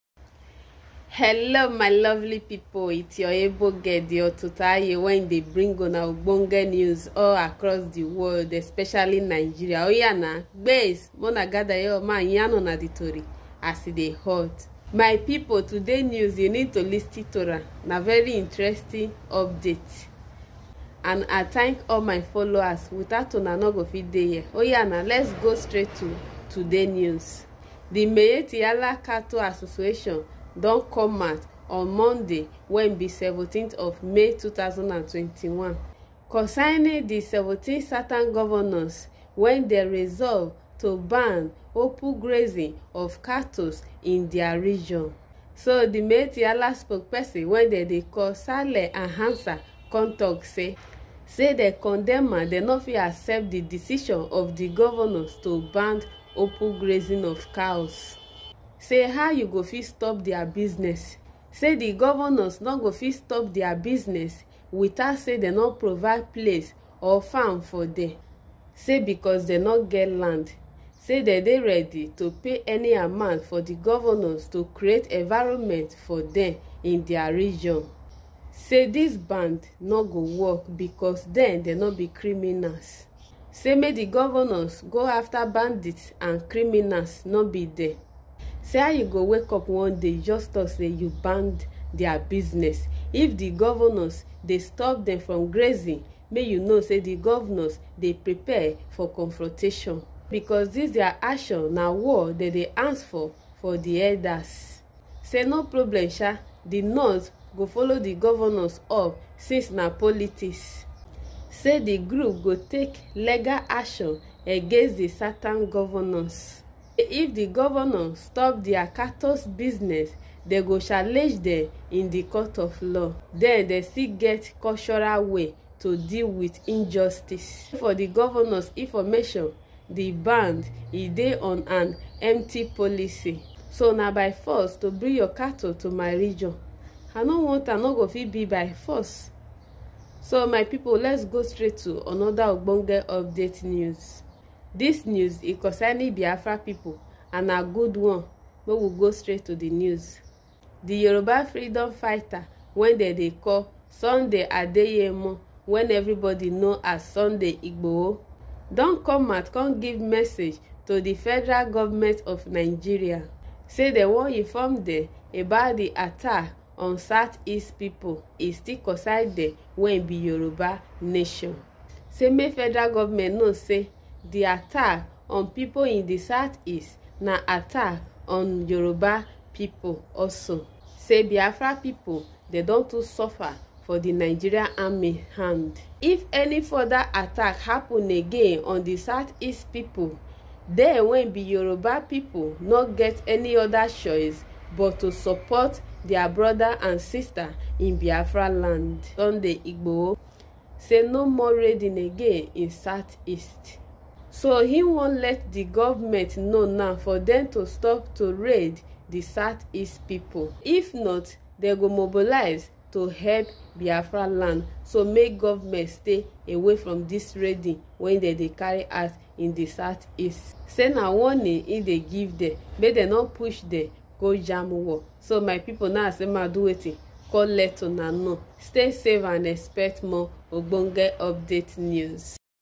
UPDATE: We'll Repel Against Further Attacks By Nigeria Army On South-East People Says Sunday Igboho
We'll Repel Against Further Attacks By Nigeria Army On South-East People Says Sunday Igboho / Southern Governors Declaration Of War On Herders On Ban Of Open Grazing Says Miyetti Allah. (This page does not support violence) 18th May 2021.